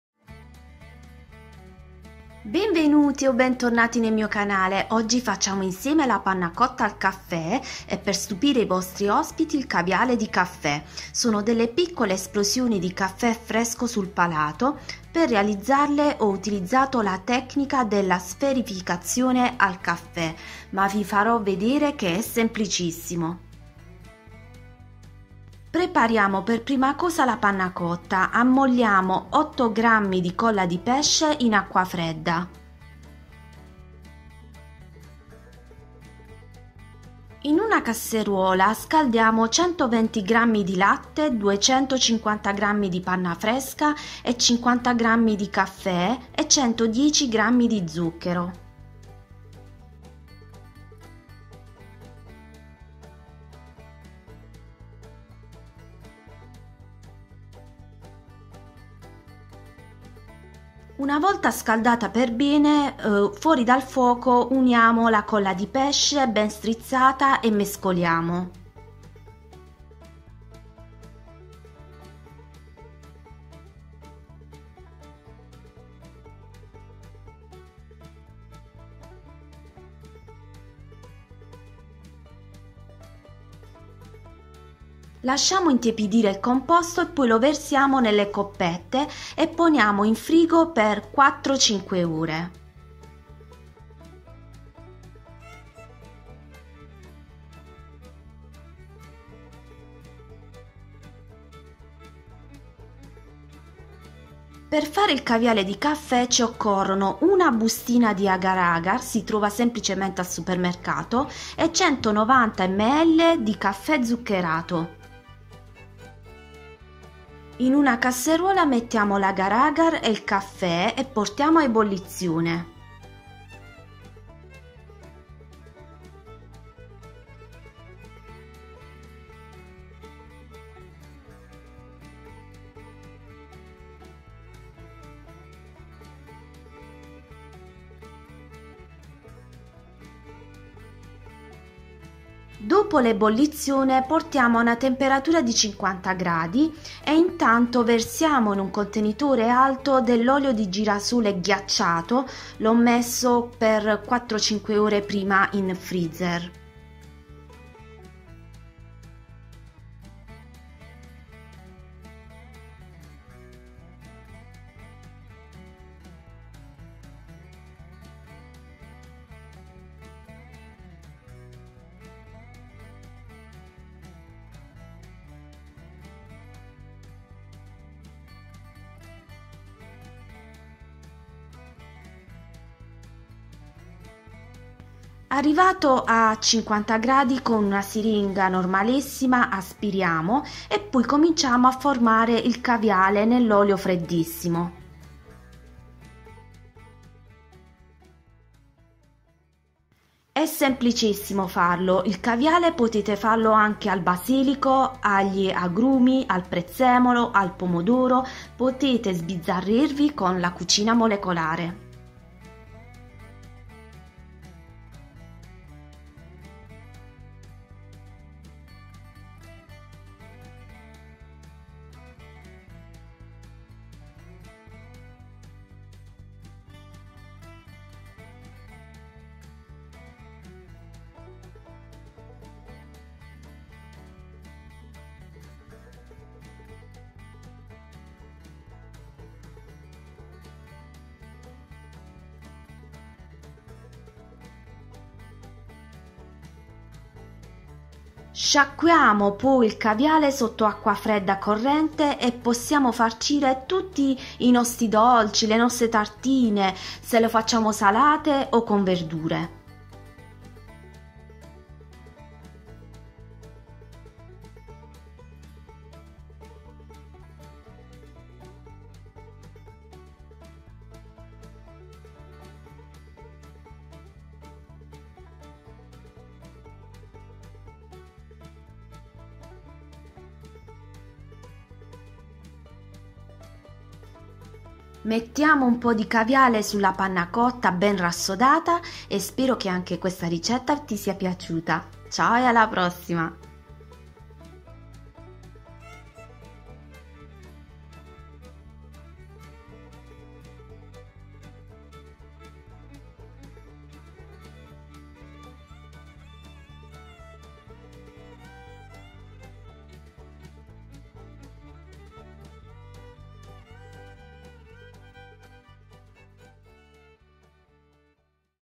Panna cotta al caffè e caviale di caffè - Dolce al cucchiaio
Oggi finalmente vi faccio vedere come si fa la sferificazione al caffè, con questa ricetta ho stupito la commissione d'esame per il mio diploma in enogastronomia. Il caviale di caffè potete utilizzare su tutti i dolci ma potete farlo anche al basilico, al nero di seppia, al pomodoro, al prezzemolo, agli agrumi, alla fragola, ecc...
INGREDIENTI PER LA PANNA COTTA:
120 ml di latte
250 g di panna fresca
50 g di caffè amaro
110 g di zucchero
8 g di colla di pesce
INGREDIENTI PER IL CAVIALE DI CAFFE':
2 g di agar agar (1 bustina)
190 ml di caffè zuccherato